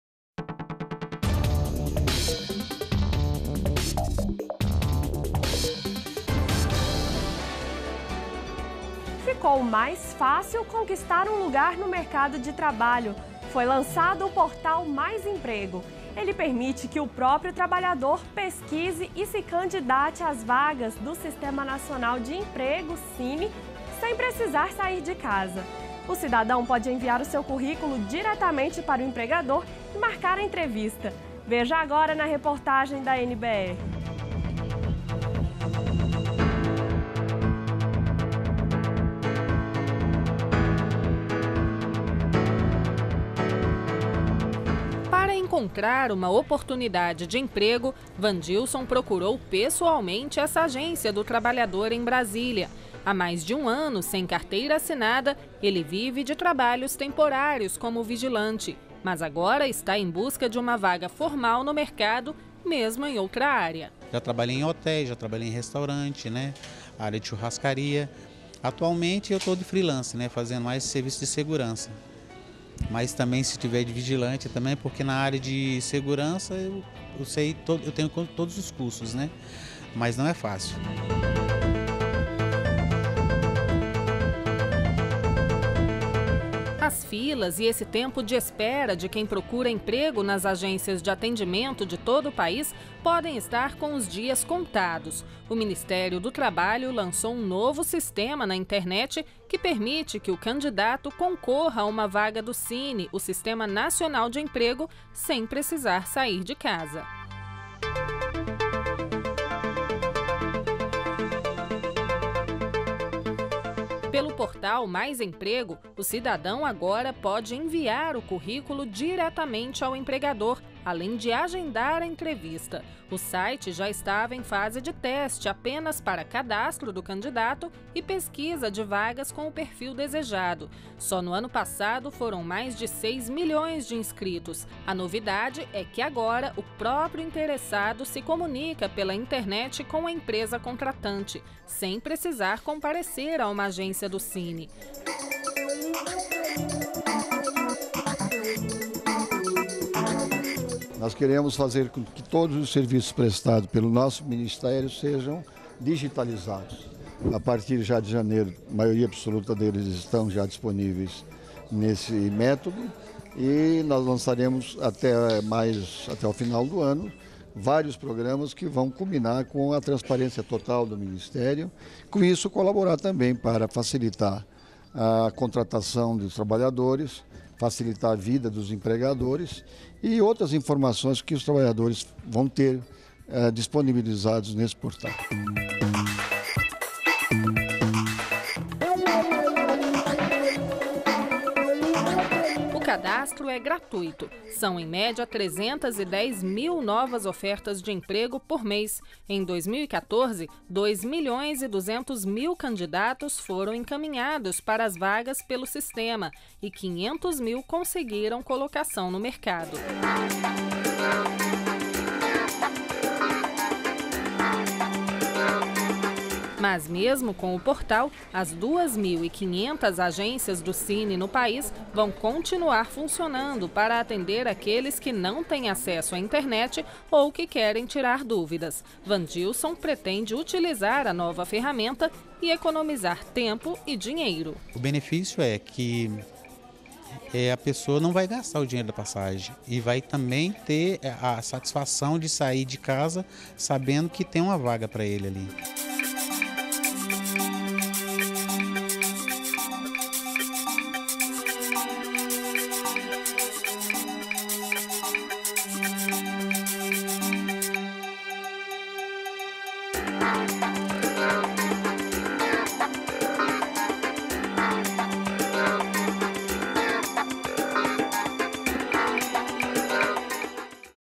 Portal Mais Emprego facilita a busca por uma vaga de trabalho
DA REDAÇÃO - 27.04.15: Os brasileiros que disputam uma vaga no mercado de trabalho contam agora com um importante aliado. O Portal Mais Emprego, lançado pelo Ministério do Trabalho e Emprego, permite que o próprio cidadão pesquise e se candidate às vagas existentes no Sistema Nacional de Emprego (Sine) sem precisar sair de casa ou depender de um intermediário.